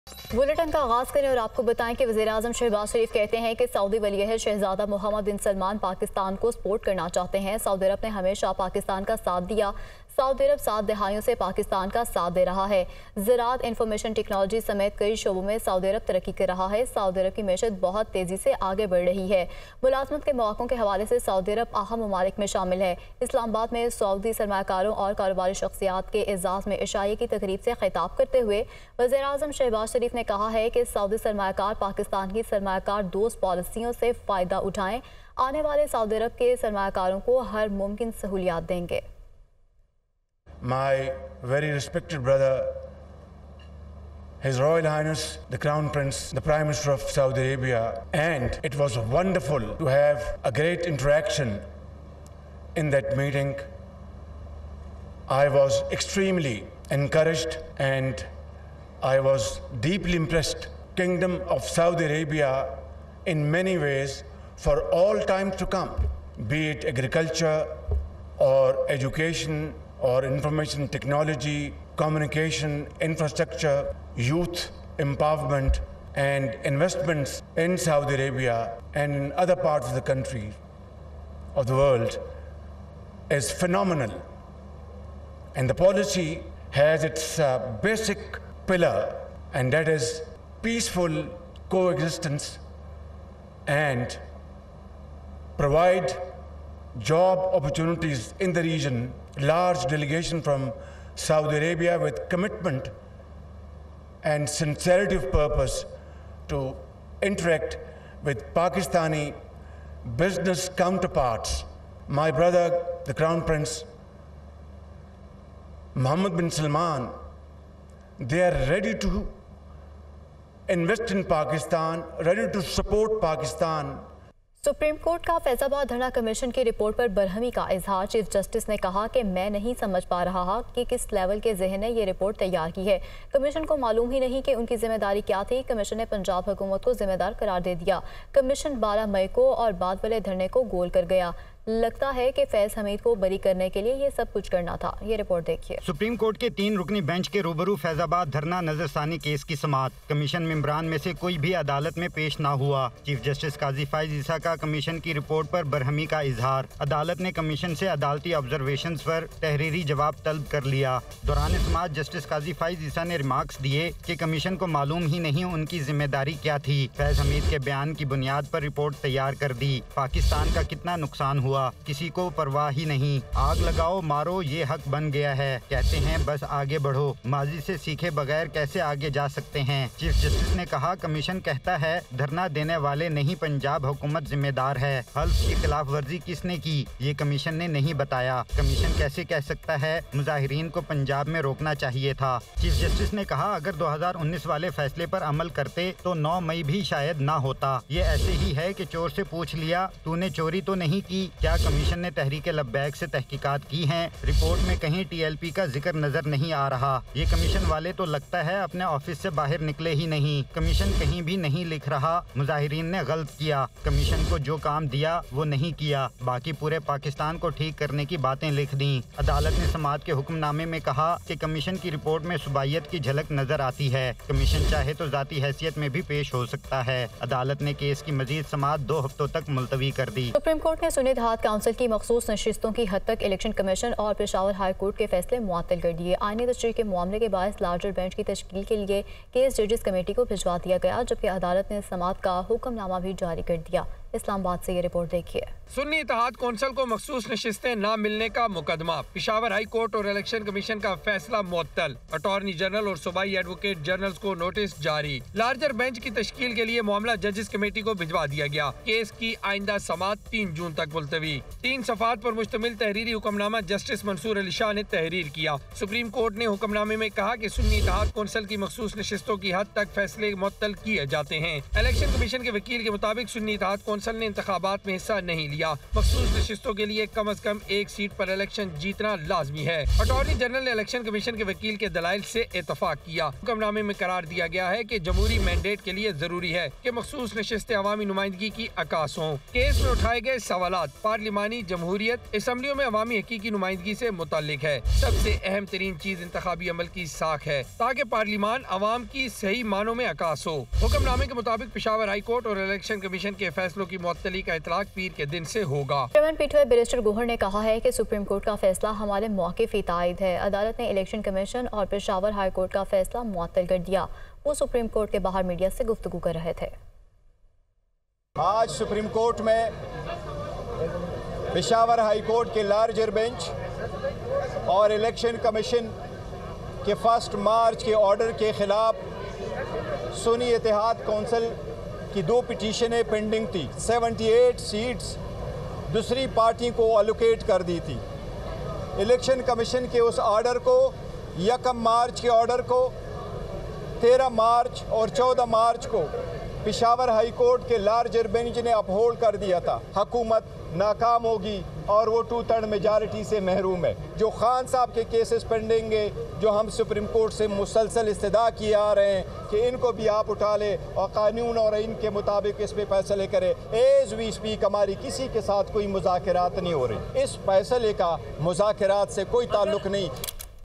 Dunya News Bulletin 12:00 AM | PM Shahbaz Sharif Big Statement Over Economy | 07 May 2024
Saudi businessmen land in Islamabad as Pakistan eyes foreign investment

OIC calls for immediate ceasefire in Gaza, urges UN to implement resolutions on Jammu and Kashmir

Wheat crisis: Kissan Ittehad announces protest demonstrations from May 10

FM Dar calls for immediate ceasefire in Gaza

Rs400bn wheat import issue storm in a teacup: Kakar

Jaffar Mandokhail to take oath as Balochistan governor today

Naqvi announces $100,000 cash prize for each player if team wins T20 World Cup

Dar expresses Pakistan's desire to increase cooperation with Yemen

 to the official channel of Dunya News. Pakistan’s Top News Network delivering the latest from around the globe.